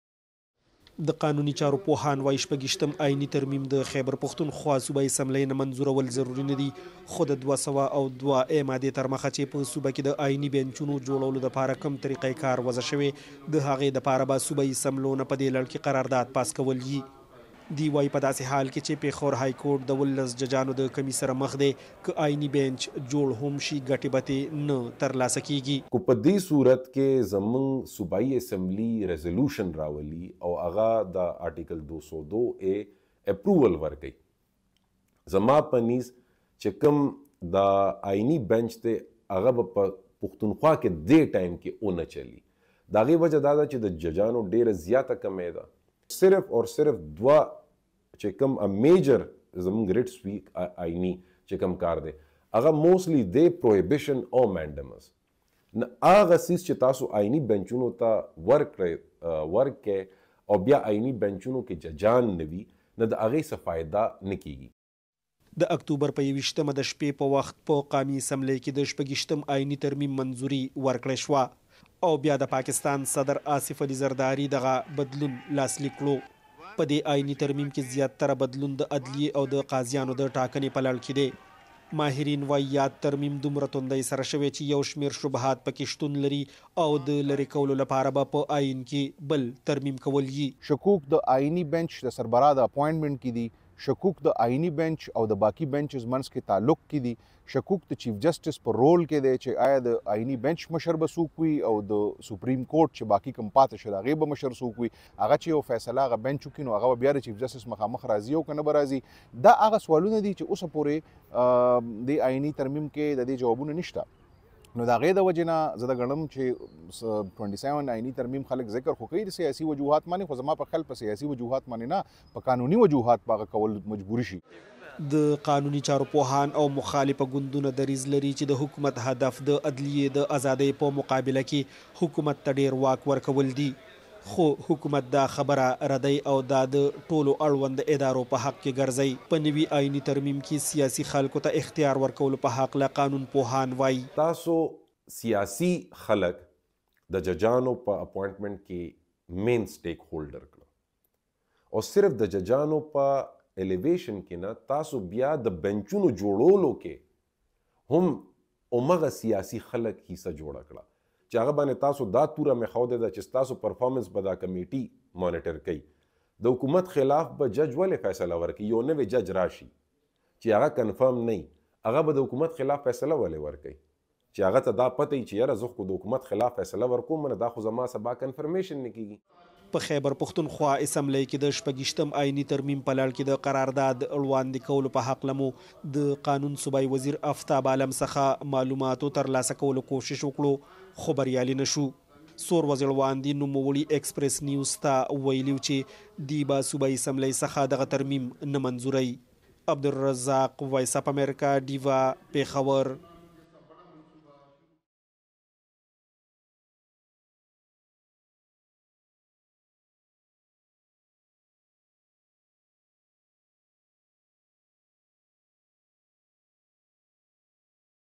په پېښور هايي کورټ کې د اېیني څانګې جوړول به ګټې وه نه لري: قانون پوهان
په پښتونخوا کې قانون پوهان وايي د ایالت په لوې عدالت کې د اېیني څانګې جوړول به ګټې وه نه لري ځکه دغلته د وړاندې نه د قاضیانو کمی دی. دوي زیاتوي په تادۍ د ۲۶مه اېیني ترمیم منظوریدو هم په دغې شکونه نور زیات کړي او پکار ده دغې لرې کړی شي. نور حال عبدالرزاق وړاندې کوي.